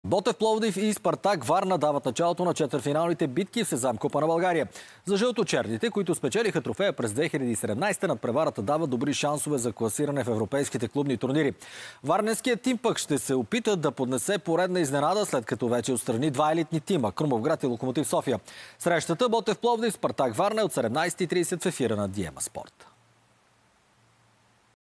Ботев Пд и Спартак Вн дават начало на 1/4-финалите за купата
Ботев Пловдив и Спартак Варна дават началото на четвърфиналните битки в SESAME Купа на България. За жълто-черните, които спечелиха трофея през 2017 година надпреварата,
дава добри шансове за класиране в европейските клубни турнири.